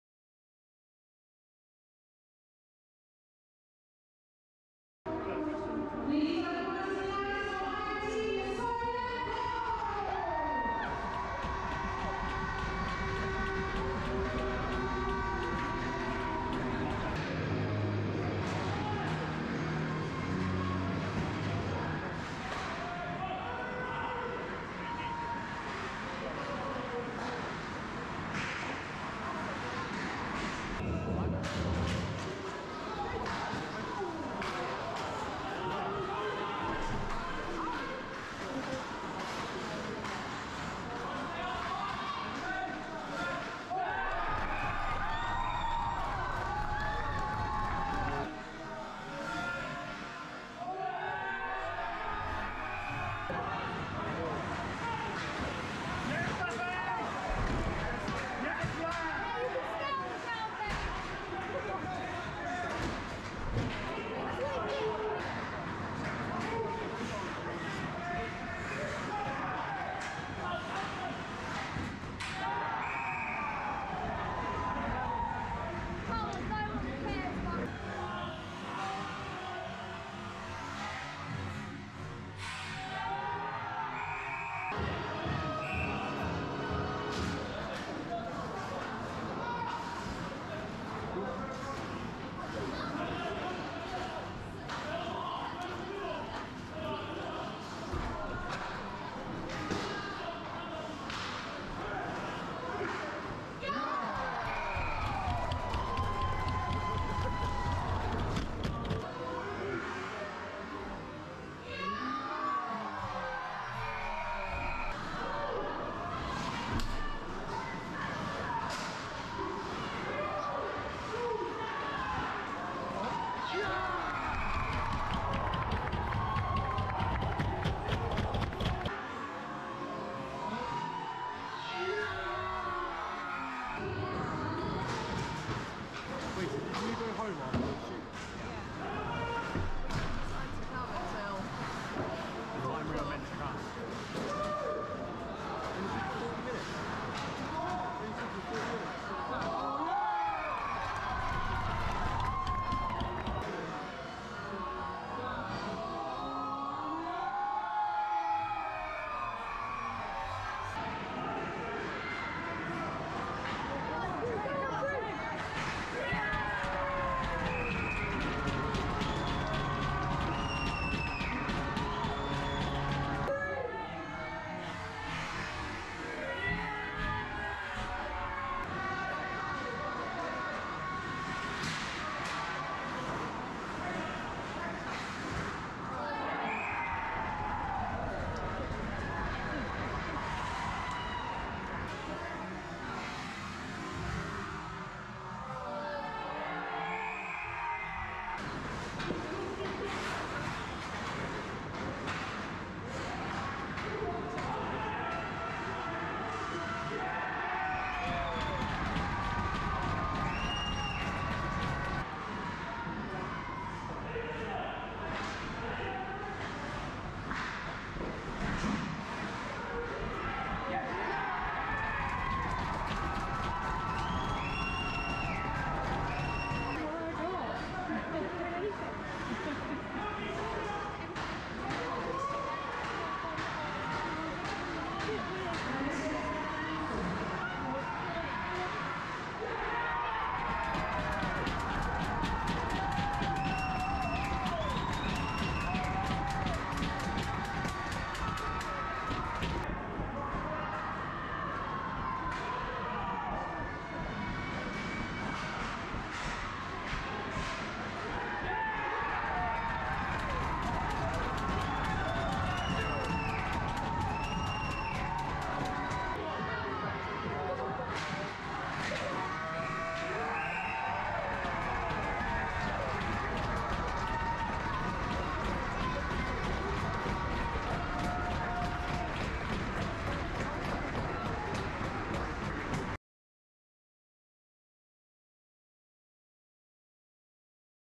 Solent Devils Vs Slough Jets
Highlights of the NIHL 1 South Cup game, Solent Devils vs Slough Jets 24th September 2022.